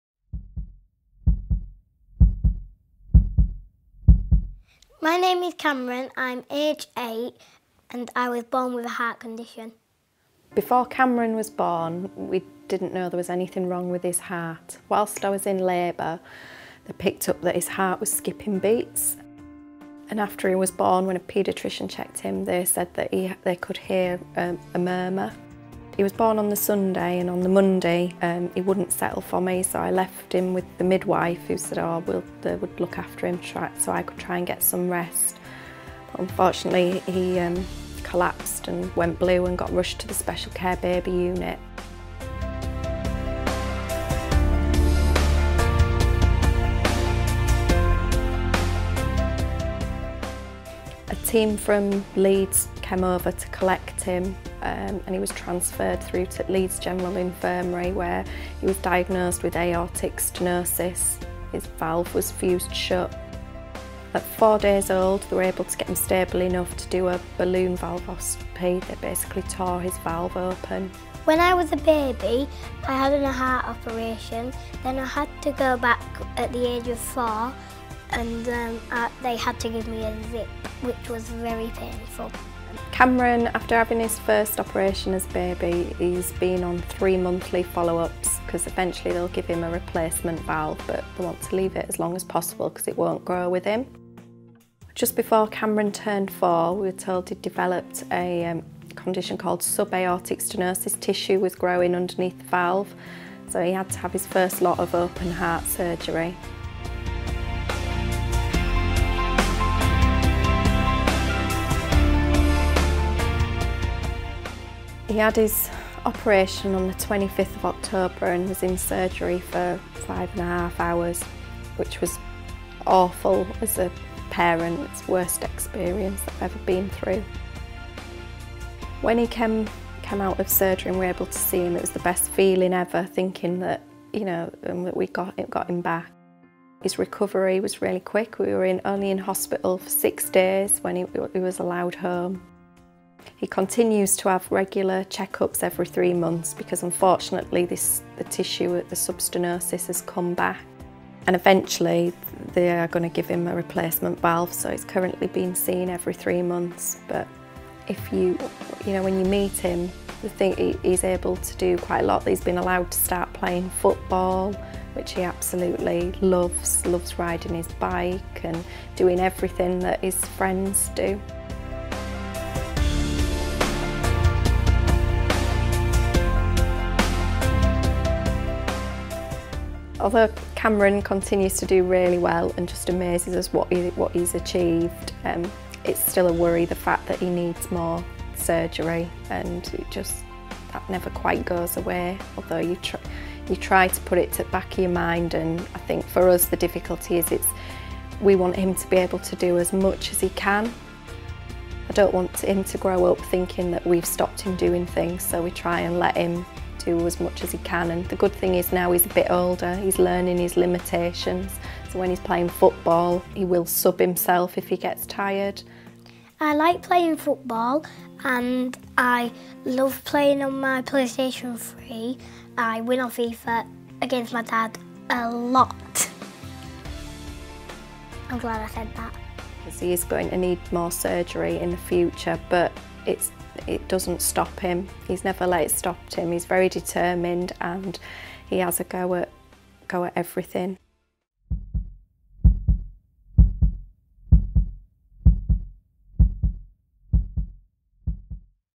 Cameron's story: Living with a heart condition
Cameron and his mum tell us what it's like growing up with a serious heart condition, and talk about his open heart surgery.